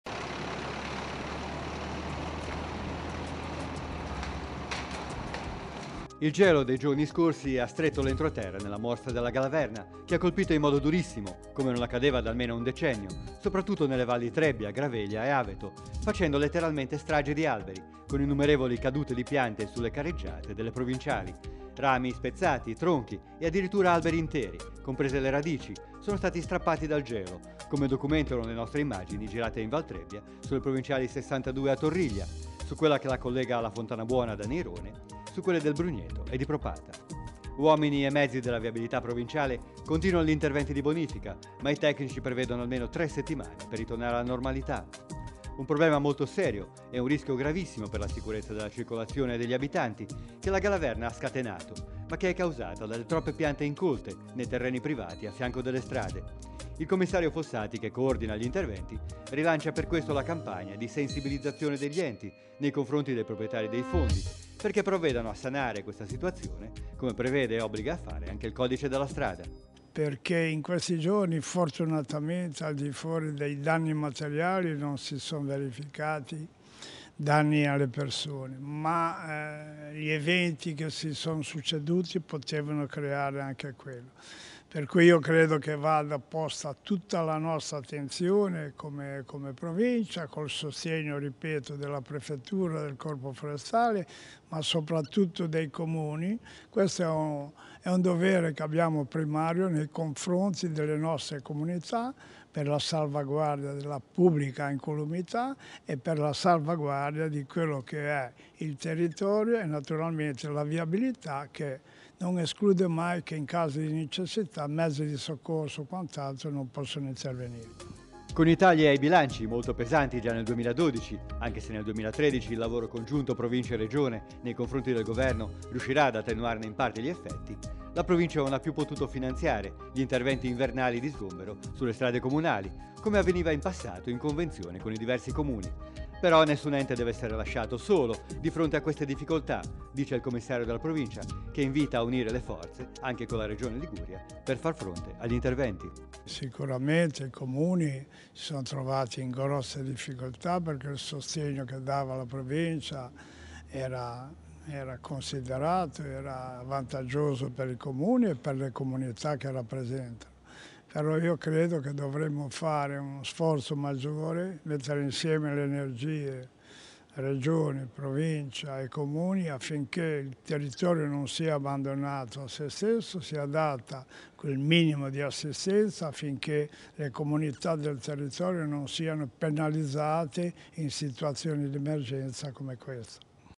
La galaverna fa strage di alberi sulle provinciali
Il gelo dei giorni scorsi ha stretto l'entroterra nella morsa della galaverna, che ha colpito in modo durissimo -- come non accadeva da almeno un decennio - soprattutto nelle valli Trebbia, Graveglia e Aveto, facendo letteralmente strage di alberi, con innumerevoli cadute di piante sulle carreggiate delle provinciali. Rami spezzati, tronchi e addirittura alberi interi, comprese le radici, sono stati strappati dal gelo -- come documentano le nostre immagini girate in Val Trebbia, sulle provinciali 62 a Torriglia, su quella che la collega alla Fontanabuona da Neirone, su quelle del Brugneto e di Propata. Uomini e mezzi della viabilità provinciale continuano gli interventi di bonifica, ma i tecnici prevedono almeno tre settimane per ritornare alla normalità. Un problema molto serio e un rischio gravissimo per la sicurezza della circolazione e degli abitanti che la galaverna ha scatenato, ma che è causata dalle troppe piante incolte nei terreni privati a fianco delle strade. Il commissario Fossati, che coordina gli interventi, rilancia per questo la campagna di sensibilizzazione degli enti nei confronti dei proprietari dei fondi perché provvedano a sanare questa situazione, come prevede e obbliga a fare anche il codice della strada.

PIERO FOSSATI
Commissario straordinario della Provincia di Genova

Con i tagli, molto pesanti già nel 2012 anche se nel 2013 il lavoro congiunto Province-Regione nei confronti del governo riuscirà ad attenuarne in parte gli effetti, la Provincia non ha più potuto finanziare gli interventi invernali di sgombero sulle strade comunali, come avveniva in passato, in convenzione con i diversi Comuni. Però nessun ente deve essere lasciato solo di fronte a queste difficoltà, dice il commissario della Provincia che invita a unire le forze, anche con la Regione Liguria per far fronte agli interventi.